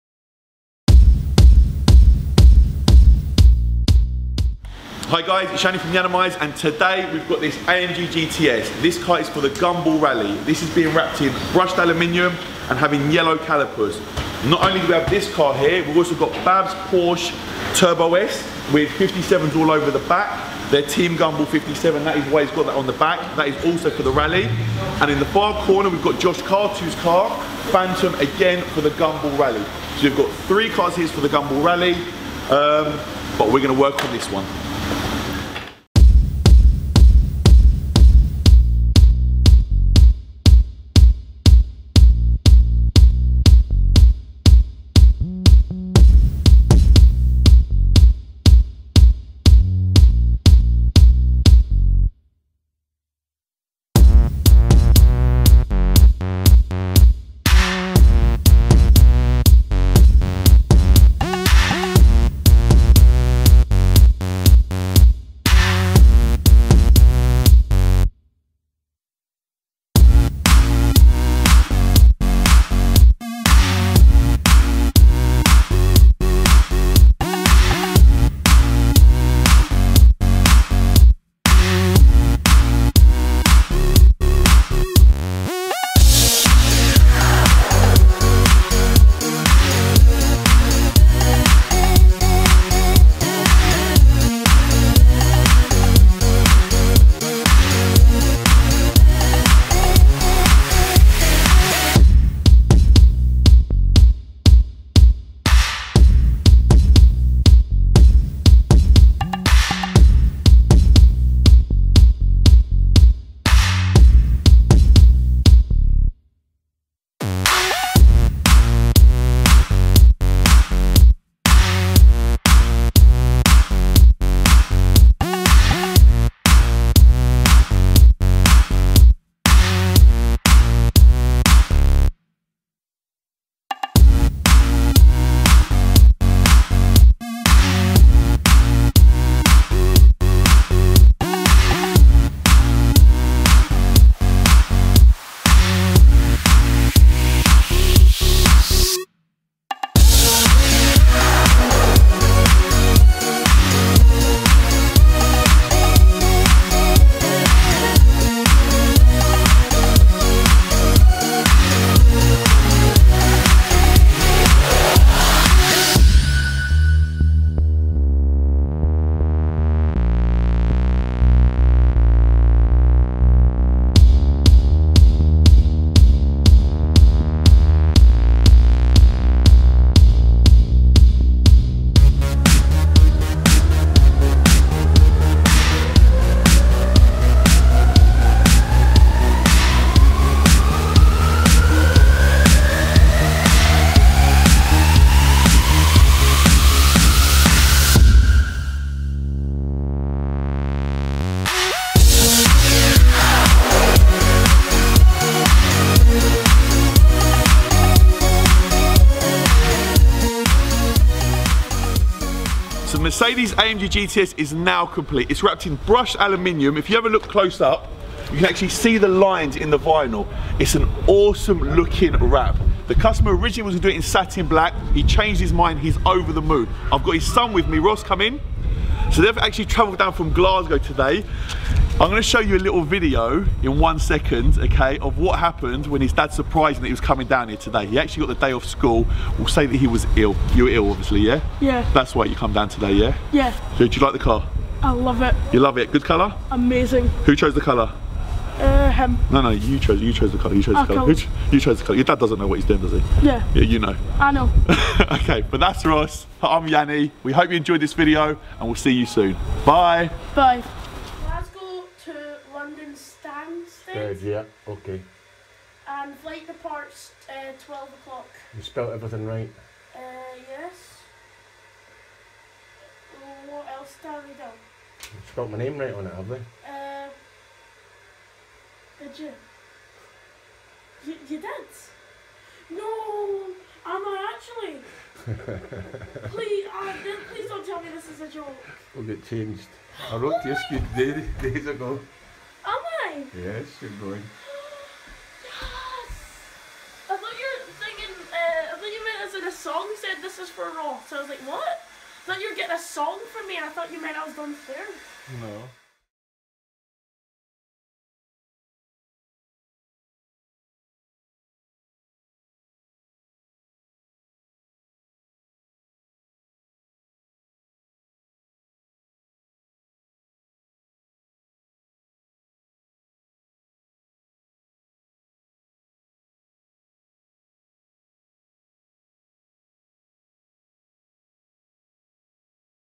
Mercedes AMG GTS wrapped Brushed Aluminum
This Mercedes AMG GTS came all the way from Glasgow to us for a colour change. It's not something customers choose a lot here, however it looks awesome with the brushed alluminium effect!